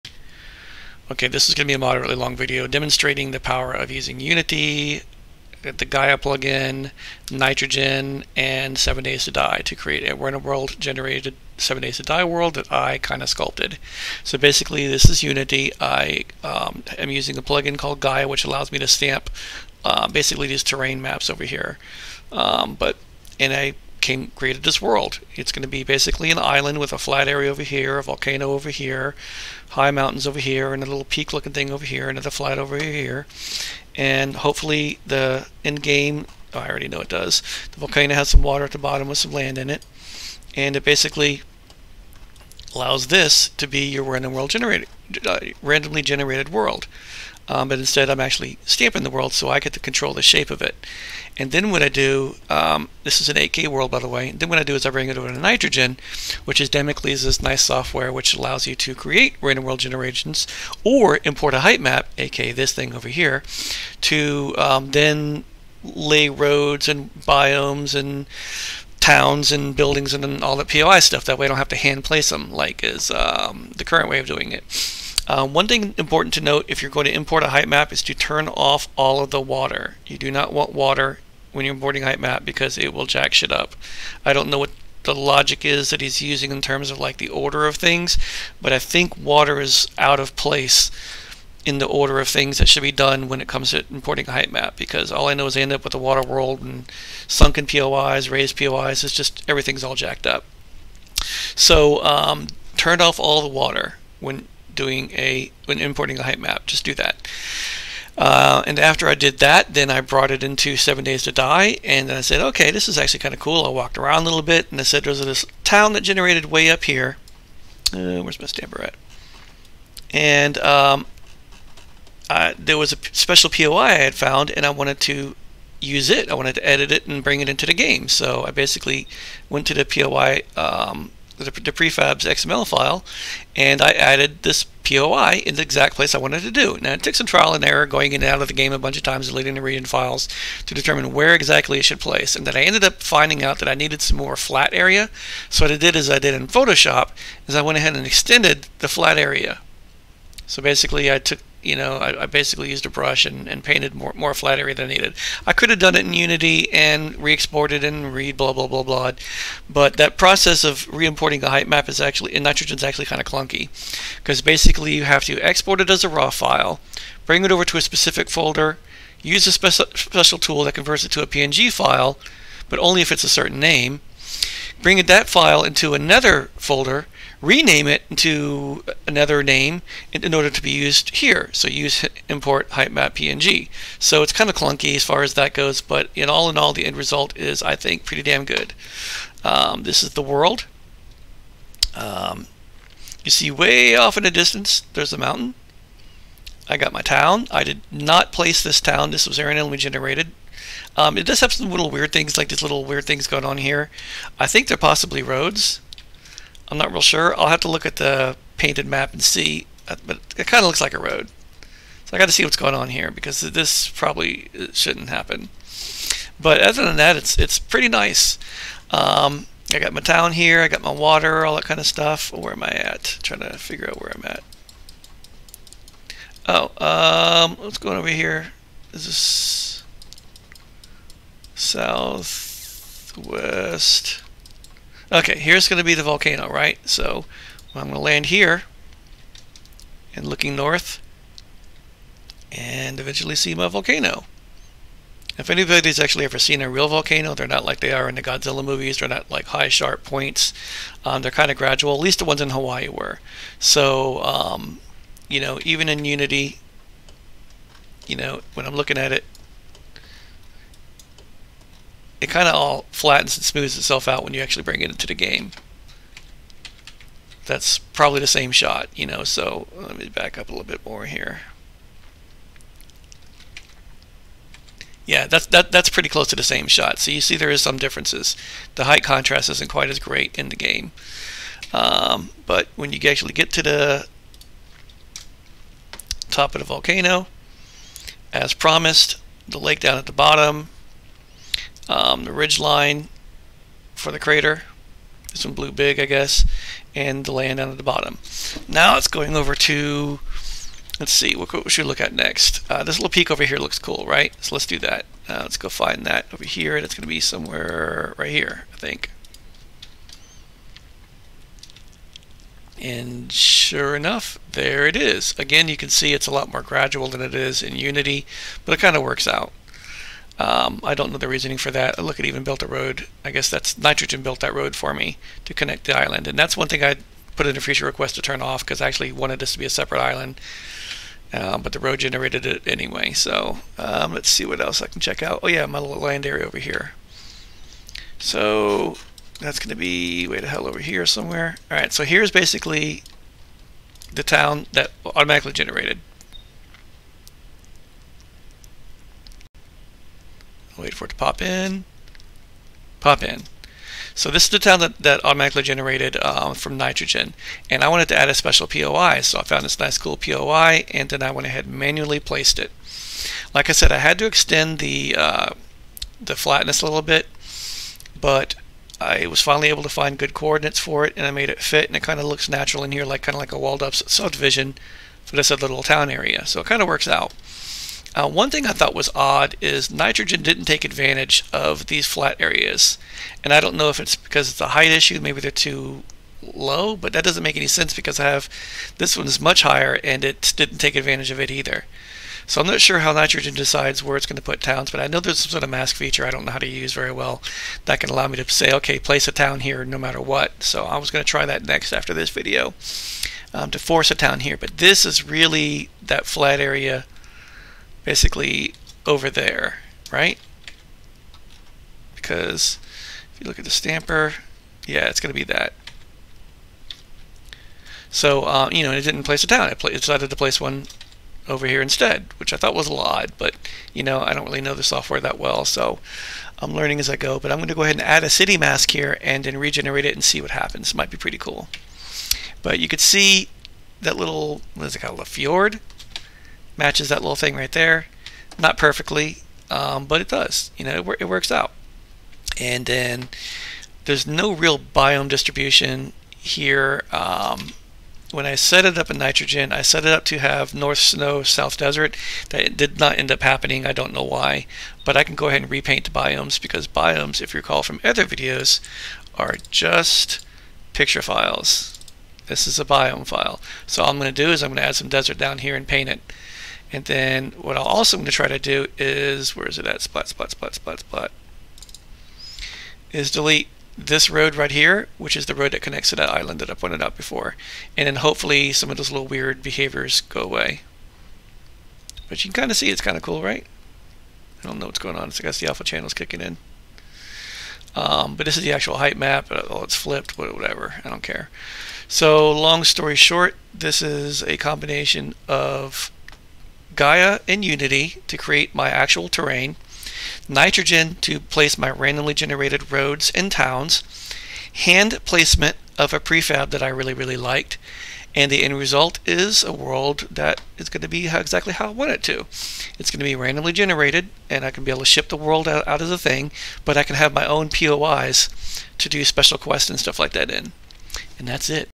7 Days to Die - A17 - Unity Gaia Nitrogen World
Using the Gaia plugin for Unity, plus NitroGen by Damocles, I'm well on my way to building a custom world in about 30 minutes.